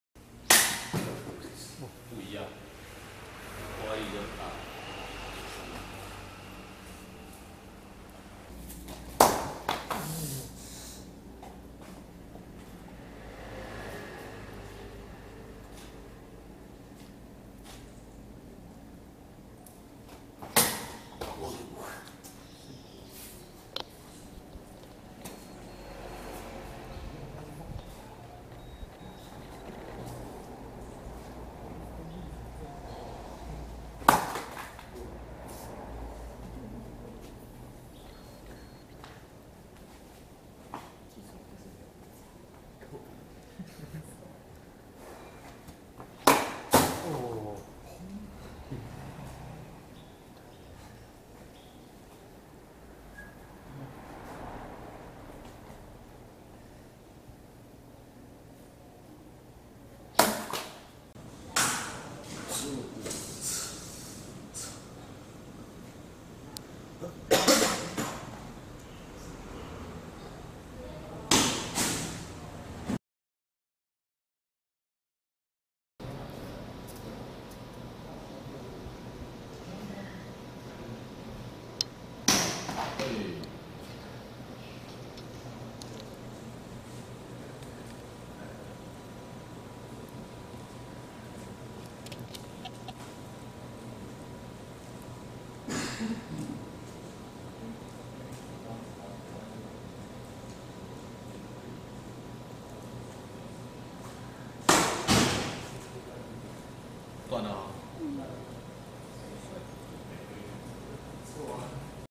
Albion Principe Mat Cutting
第一次斬草席體驗,最後一捲草蓆因斬台壞掉為無固定狀態!難度更高!
使用劍為Albion出品的Principe手半劍~

My first mat cutting experience with the new Albion Principe Bastard Sword,
the last mat was unfixed because the stand was damaged, makes it more difficult!

每個月舉辦聚會活動
提供一個平台，讓武友們能武技交流
不再局限於自身門派，實現武術無國界的競技交流！